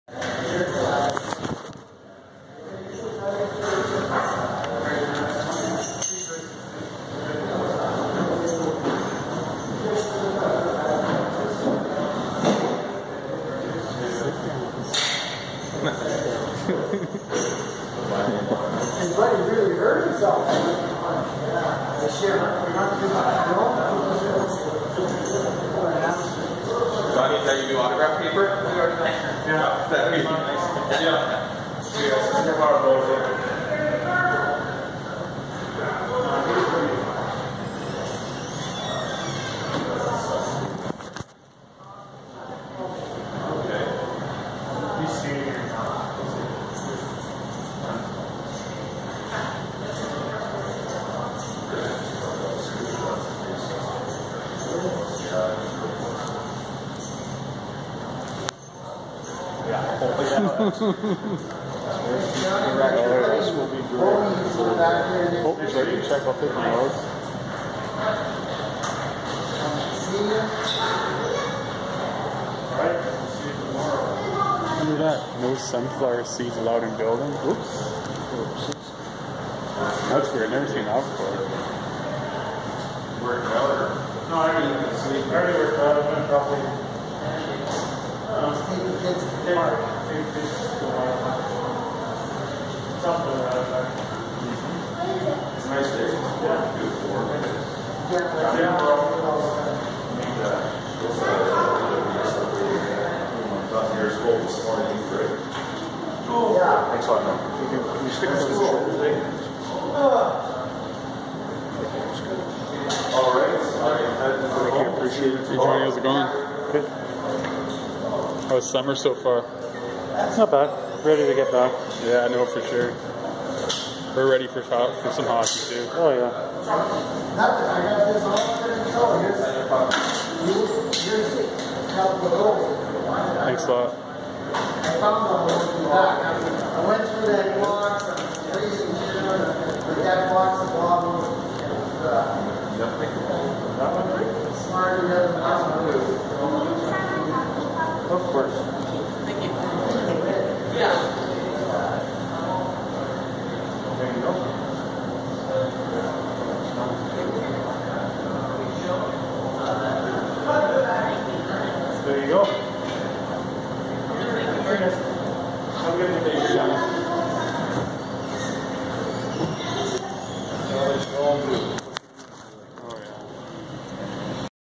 Johnny Boychuk signs autographs for fans in Edmonton after practice
Johnny Boychuk of the New York Islanders signs autographs for fans after Perry Pearns 3 on 3 practice in Edmonton